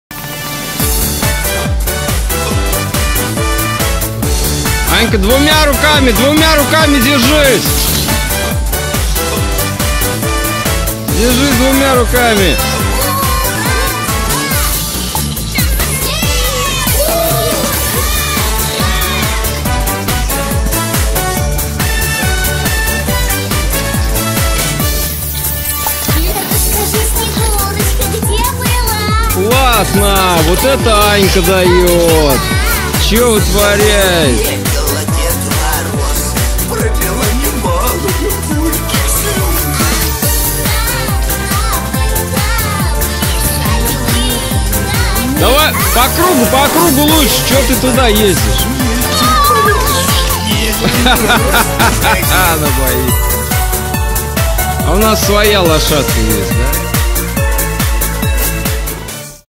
Такое только в детстве возможно.
Парк, зима, братик катает сестрёнку.